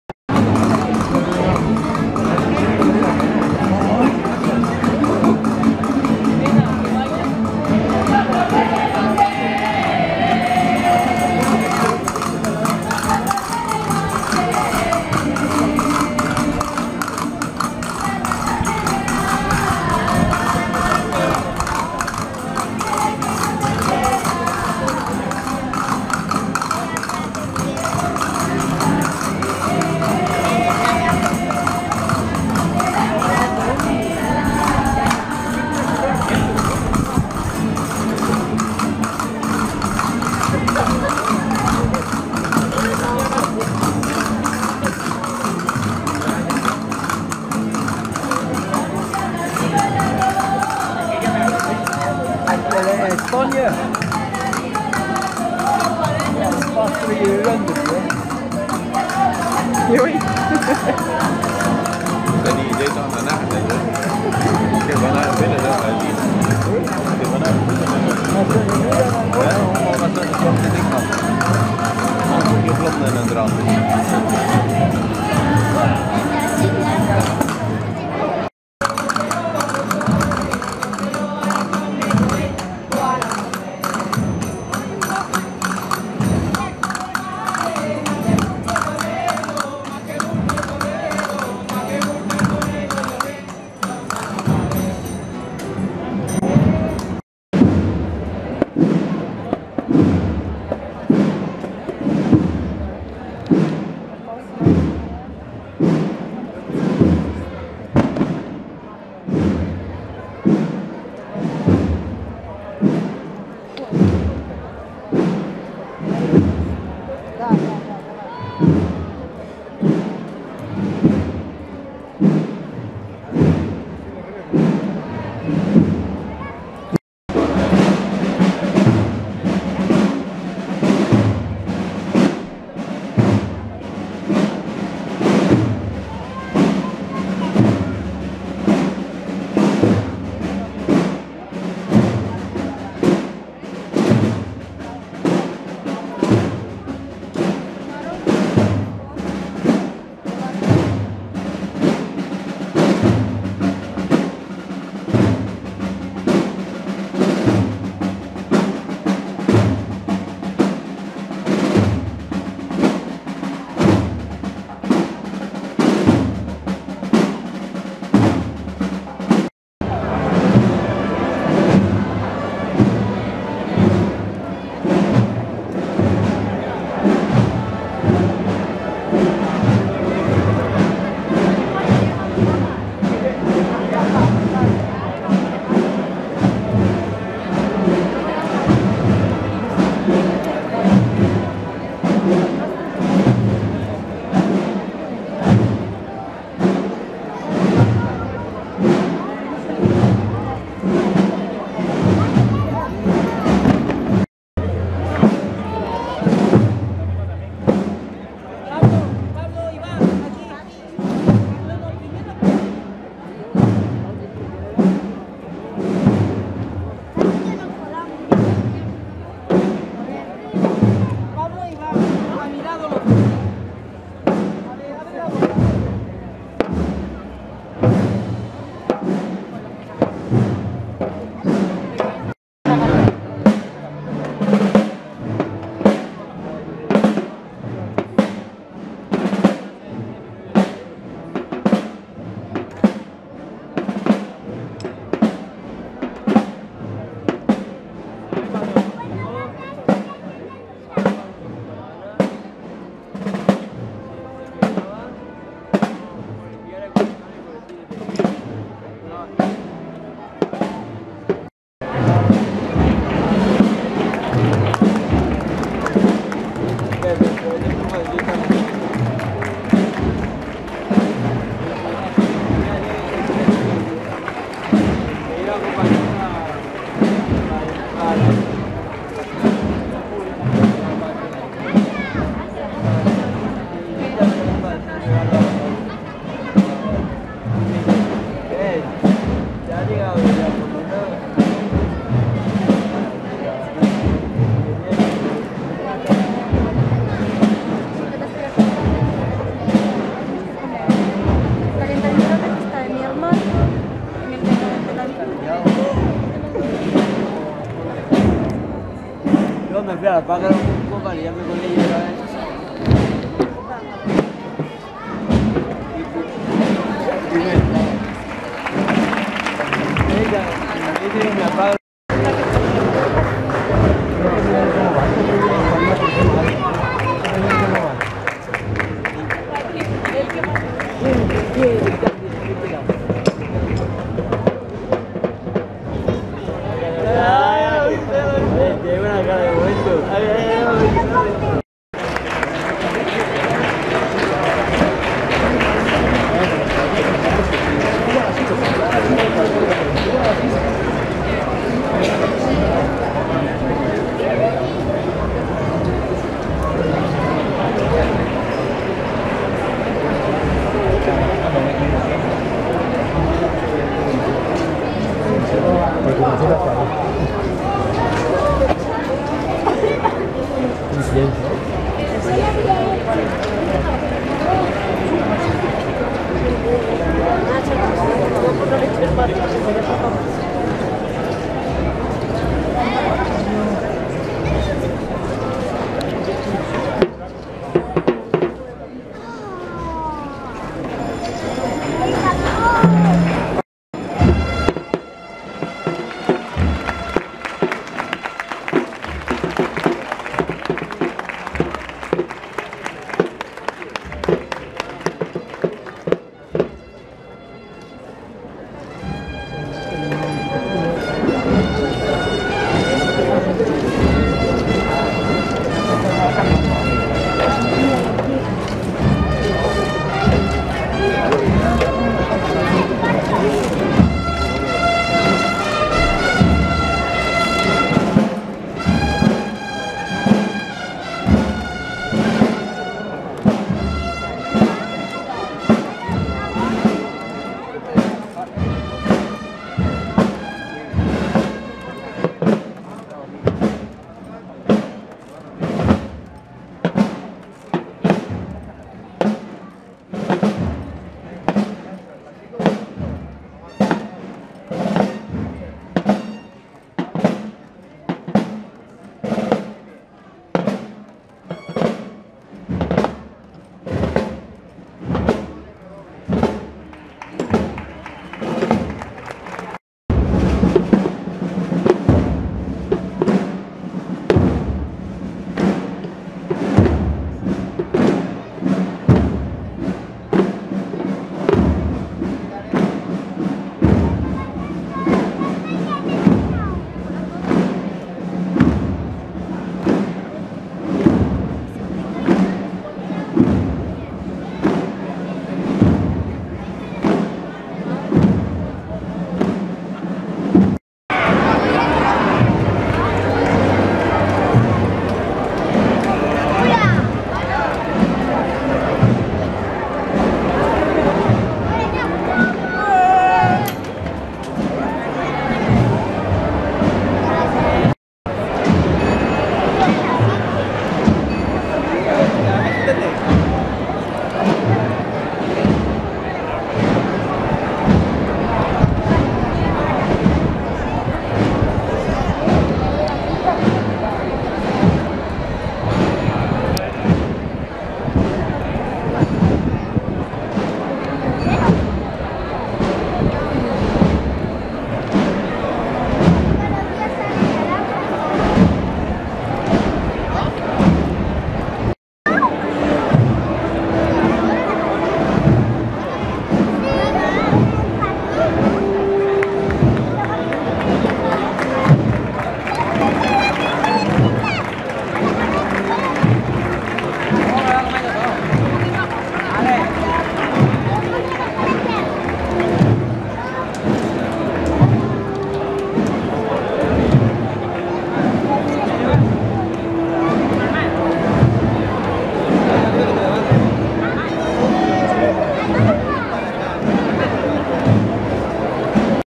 100-semsanta deel1
De Heilige week (Semana Santa) in Cartagena (Deel 1)